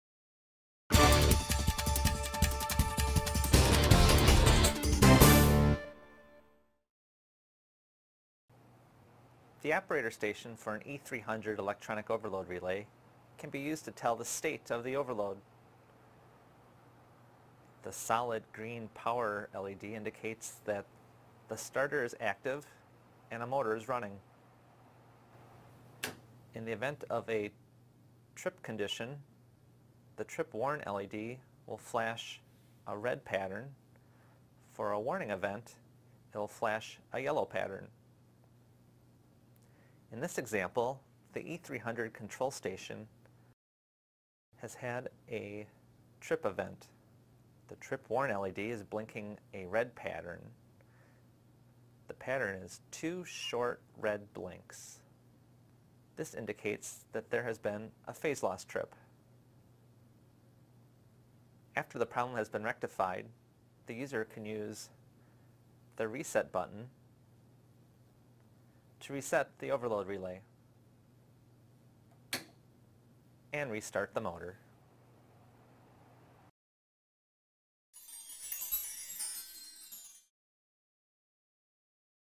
E300 Electronic Overload Relay - Using Control Station to Examine Phase Loss
A demonstration of how to use E300 Electronic Overload Relay Control Station analyze a phase loss trip.